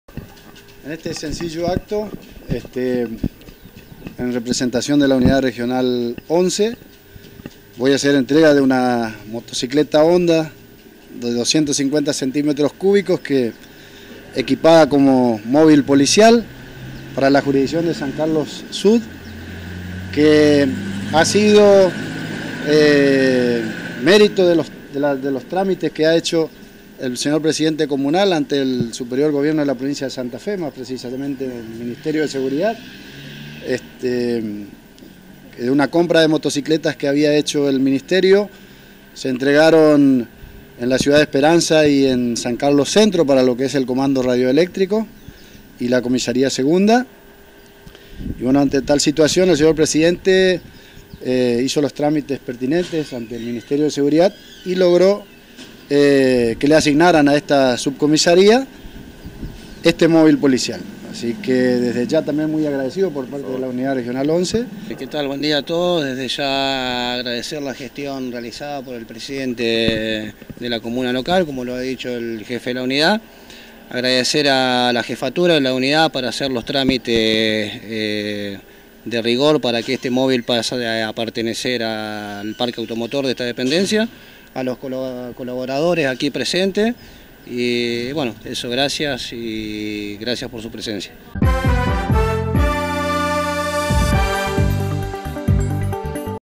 JUAN VOULLIOZ – COMISARIO GENERAL – JUAN MERLO – COMISARIO
ENTREGARON UNA NUEVA MOTOCICLETA EN LA SUBCOMISARÍA SEXTA